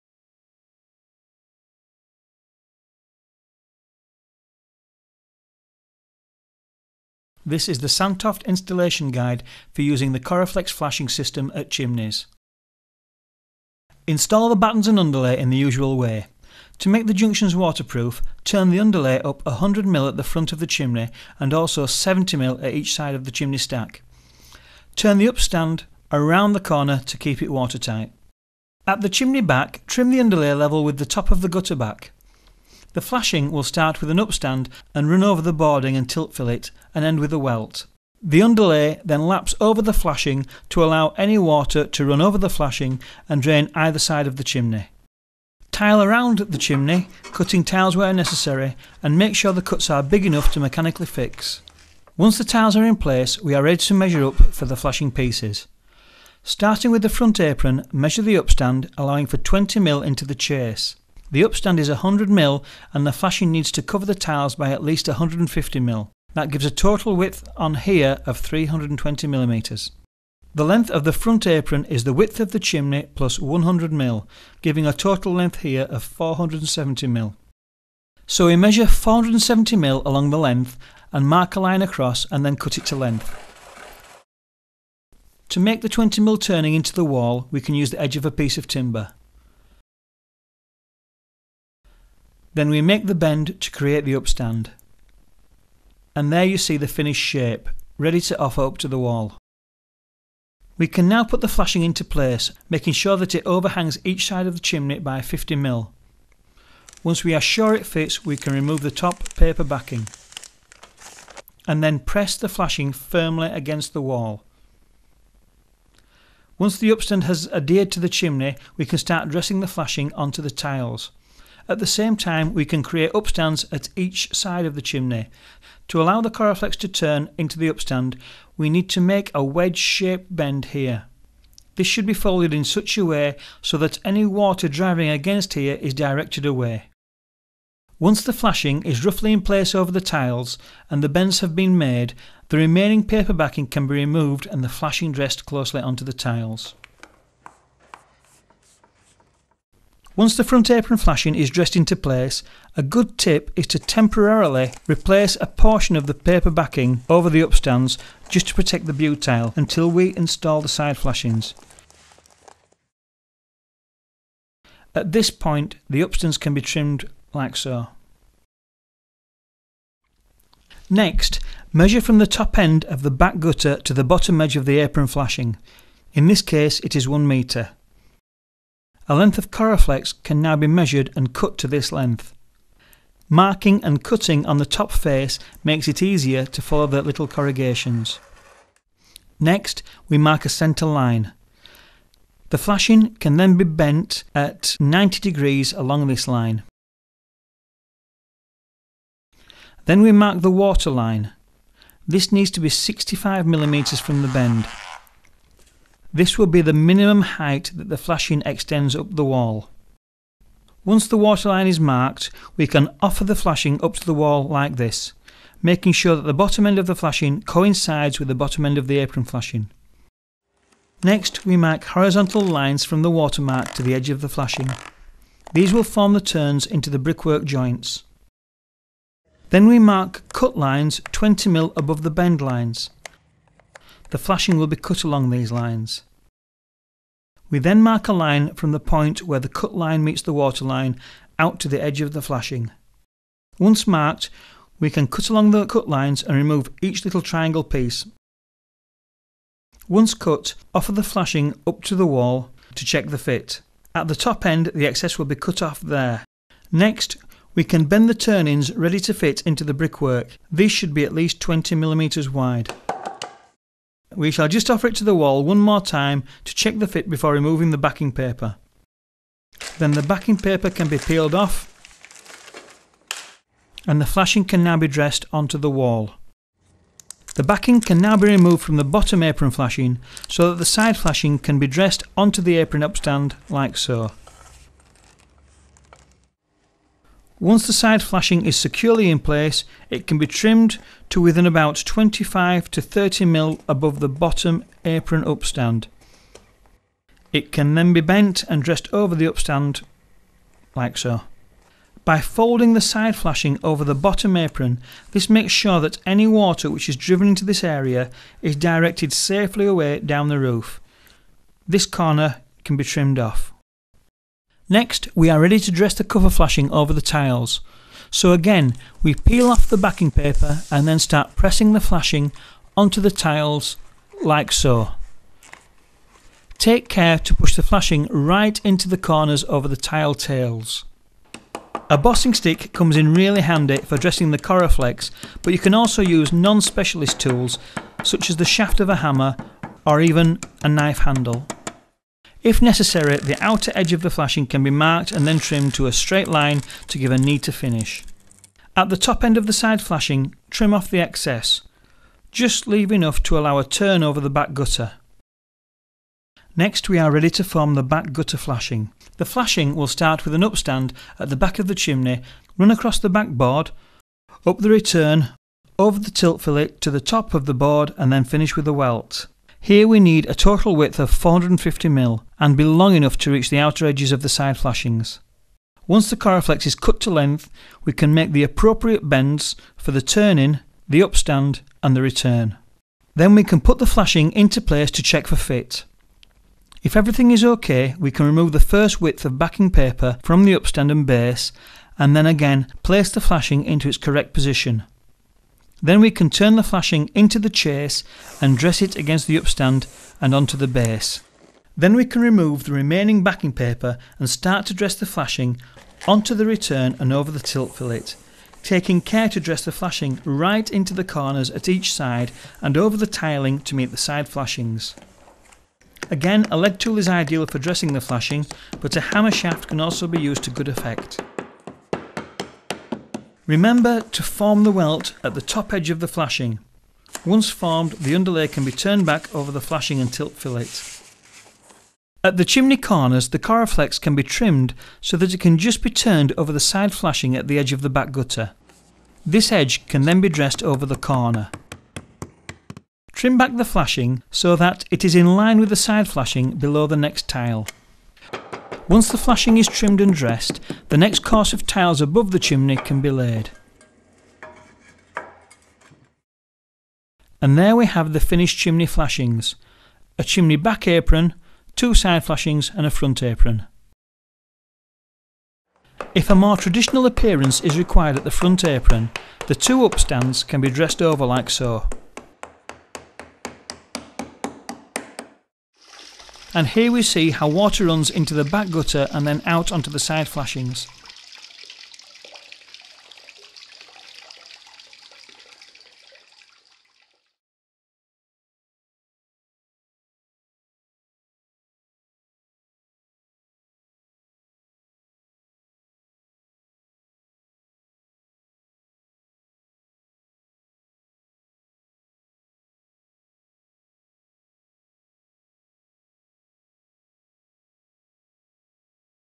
Wienerberger UK: Installation guide for Sandtoft KoraFlex
Wienerberger is the leading provider of building material solutions.

A comprehensive guide from Wienerberger showing how to install Sandtoft KoraFlex flashings. This video explains specifically how to detail the side, top and bottom flashings around a chimney.